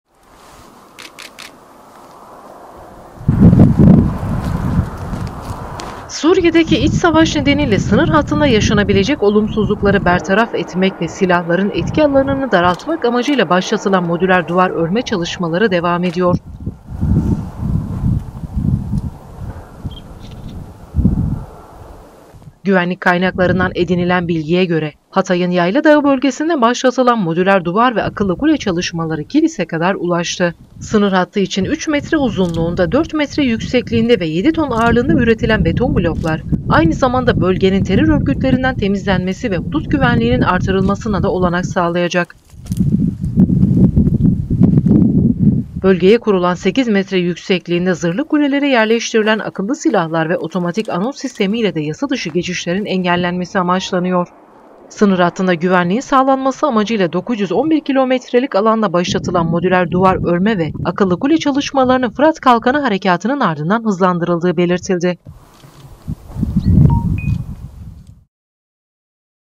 Suriye Sınırında Modüler Duvar Çalışmaları
Suriye'deki İç Savaş Nedeniyle Sınır Hattında Yaşanabilecek Olumsuzlukları Bertaraf Etmek Ve Silahların Etki Alanını Daraltmak Amacıyla Başlatılan Modüler Duvar Örme Çalışmaları Devam Ediyor.